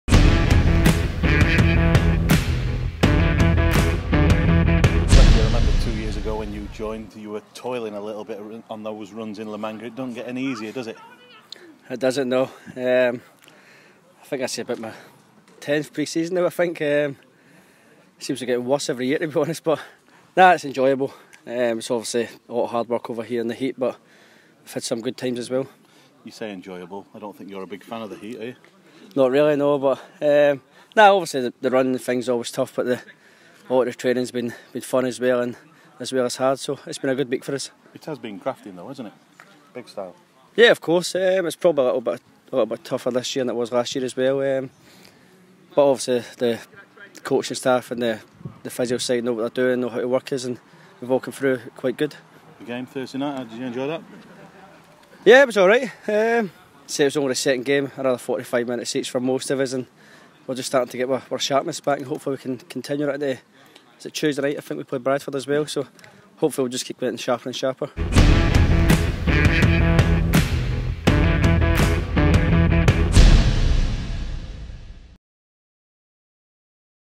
John Fleck putting the hard yards in
John Fleck is not a big fan of the heat but has been putting the hard yards in with his team-mates